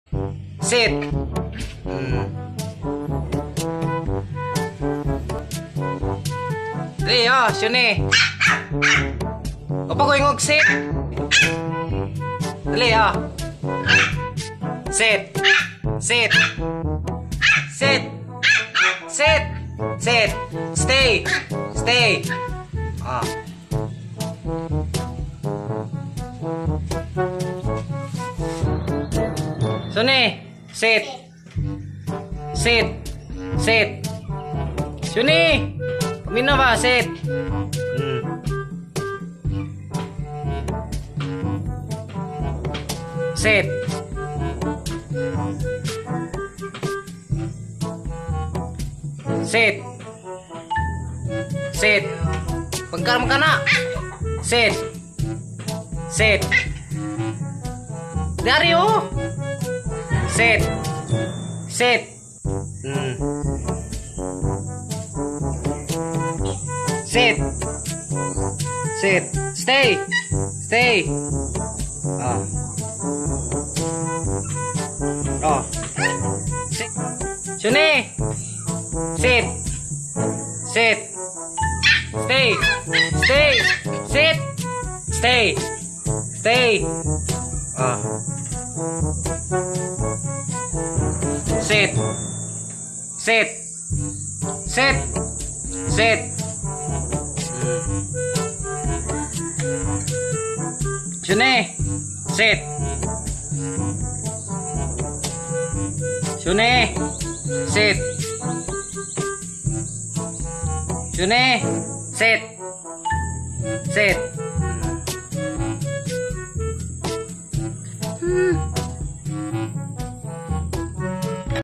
V#2..In Memory of Sionie_Aspin Puppy..🐕 w/Gee Mi Lo
This is Sionie our 2nd puppy, a local dog breed(Aspin), she's just only 2 months, unfortunately she died, she was vomitting and pooped with blood. We missed you Sionie...

YT_channel Lyn Bals